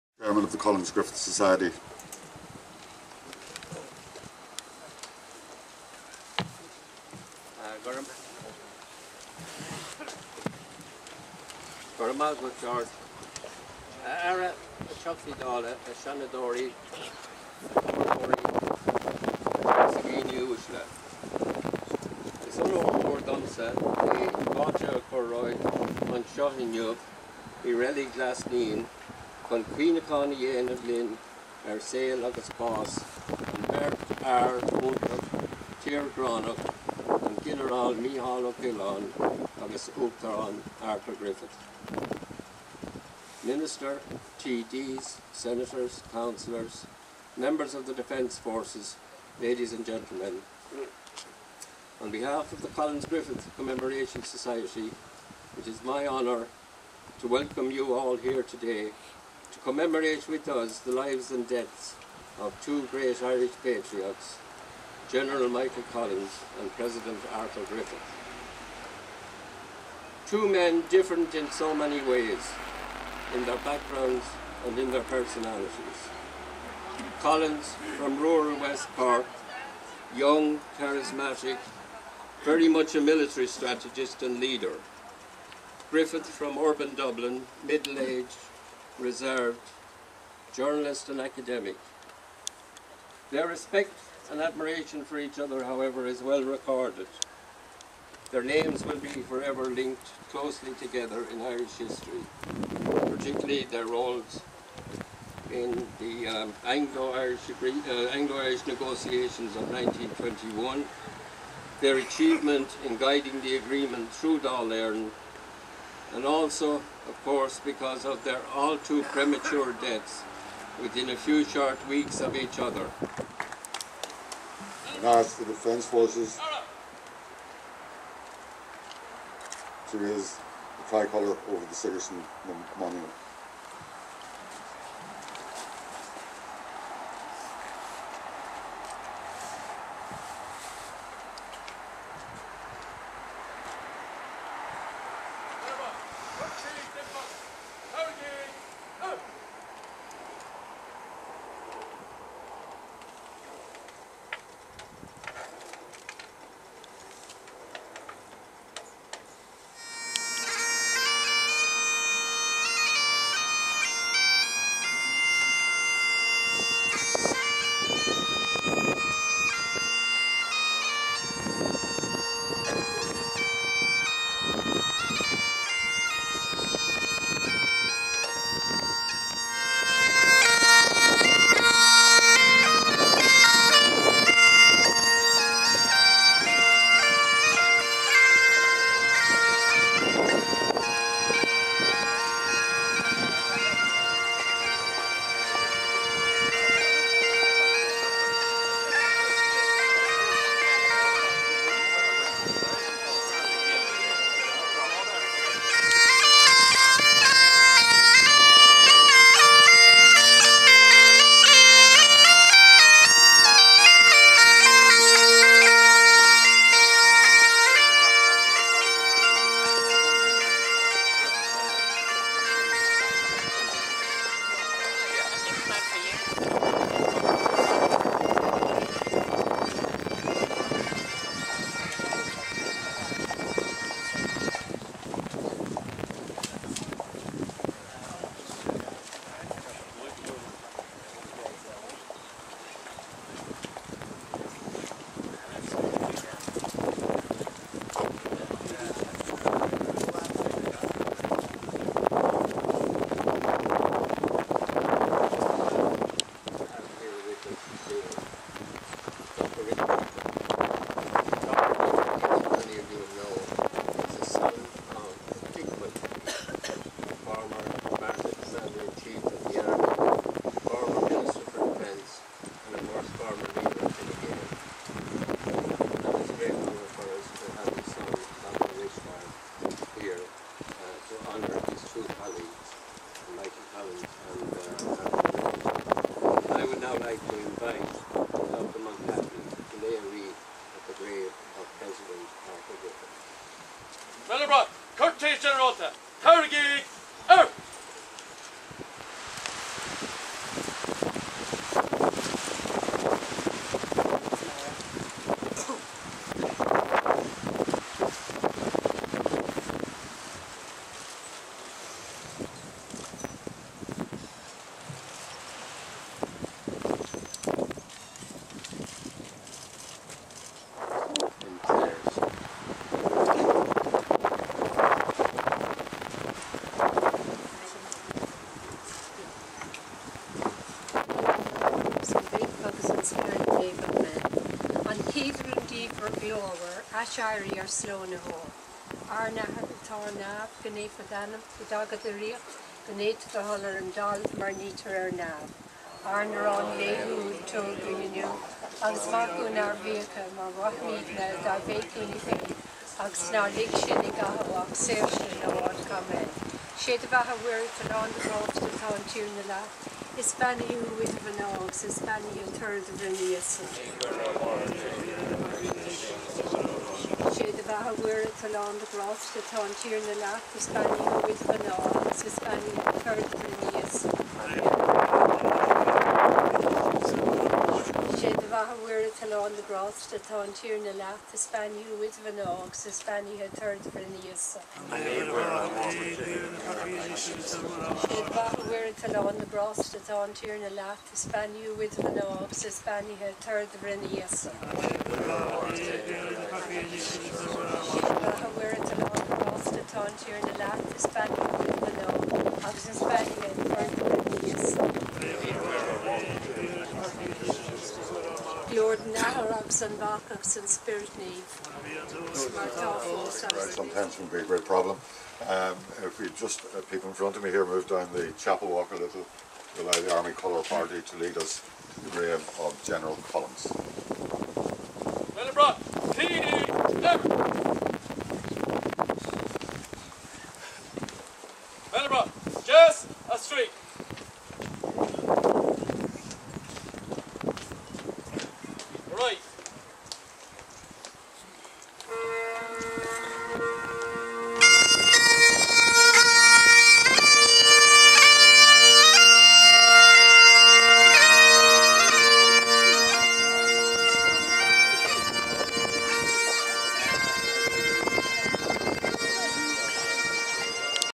Michael Collins/ Arthur Griffith commemoration 2014 part 2
The annual comemoration of the deaths of Michael Collins and Arthur Griffith held in Glasnevin cemetary, 17th August 2014. Part 2 of 3.
This shows the opening, raising of the flag and march to Arthur Griffith's grave + ceremony.